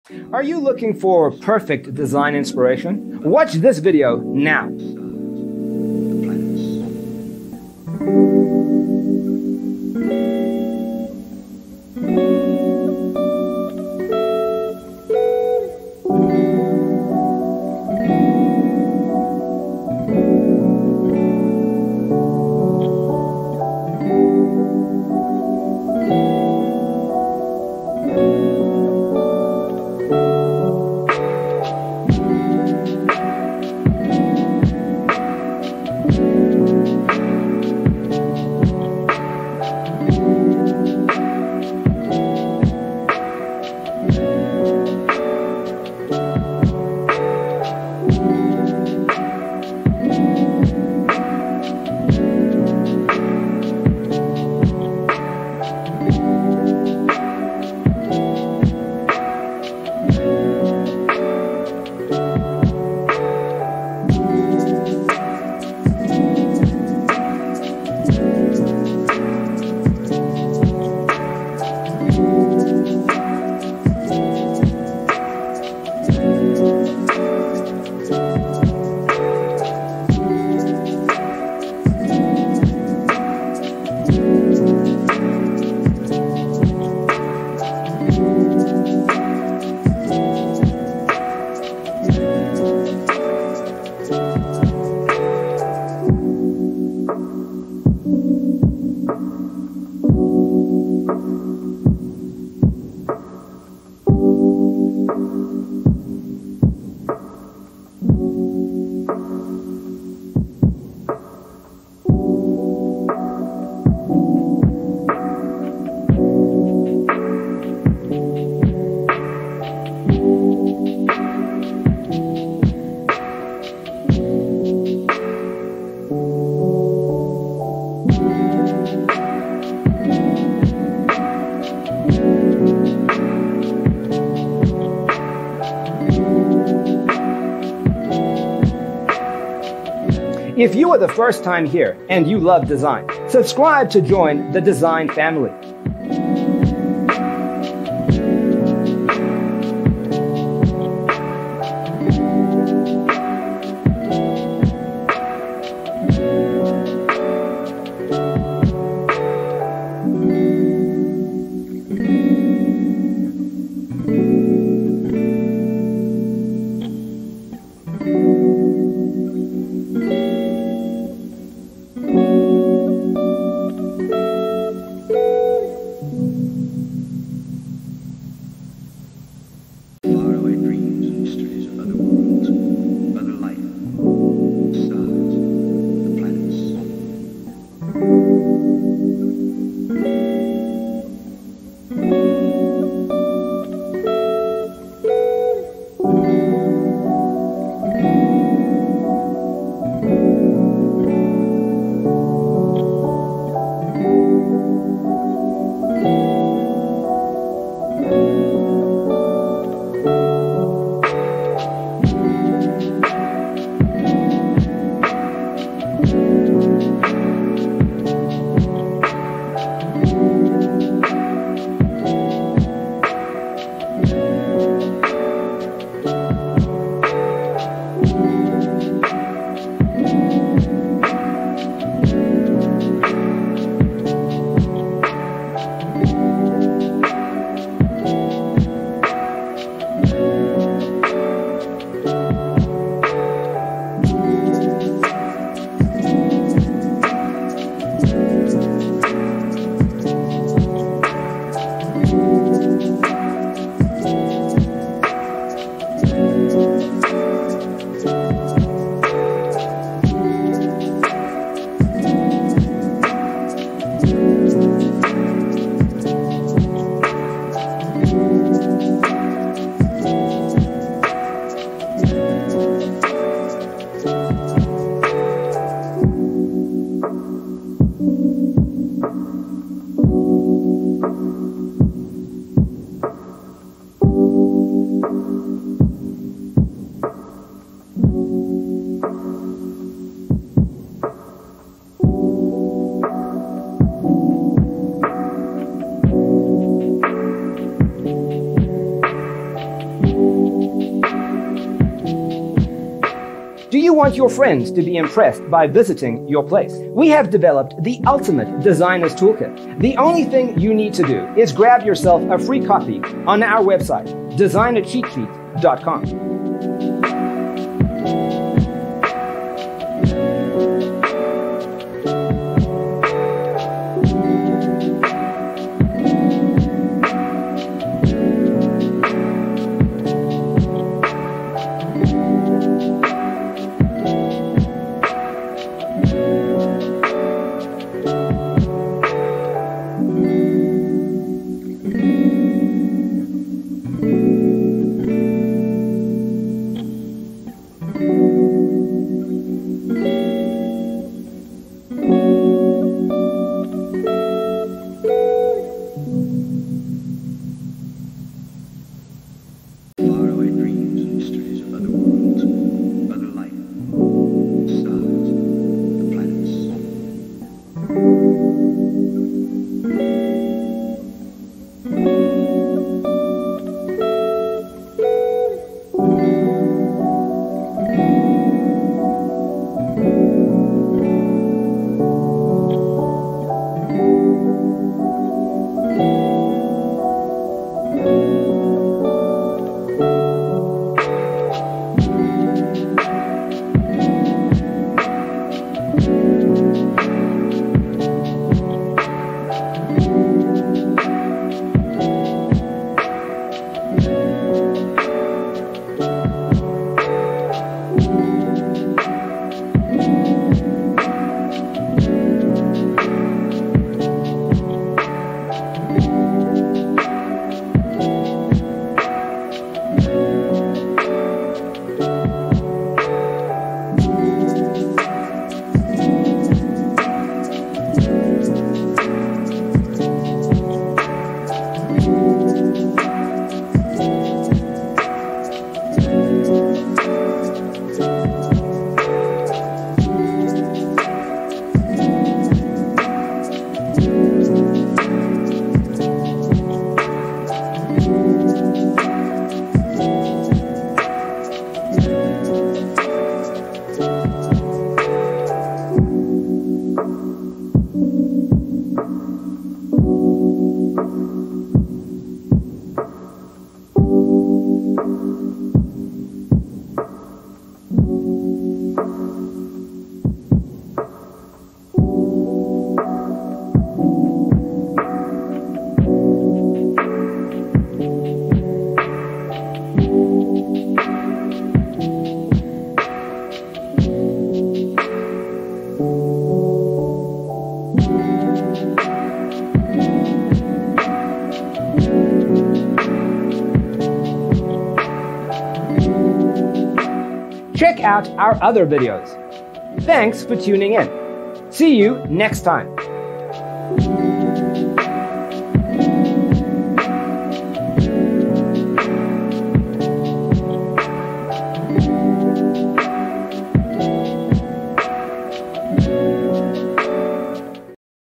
75 Asian Concrete Paver Landscaping Design Ideas You'll Love 😊
Browse Asian landscapes and gardens. Discover new Asian landscape designs and ideas to boost your homes curb appeal.

0:00:00 - Intro
0:00:12 - Asian Concrete Paver Landscaping Design Ideas You'll Love: Inspirations 1 to 25
0:03:03 - Asian Concrete Paver Landscaping Design Ideas You'll Love: Inspirations 26 to 50
0:06:07 - Asian Concrete Paver Landscaping Design Ideas You'll Love: Inspirations 51 to 75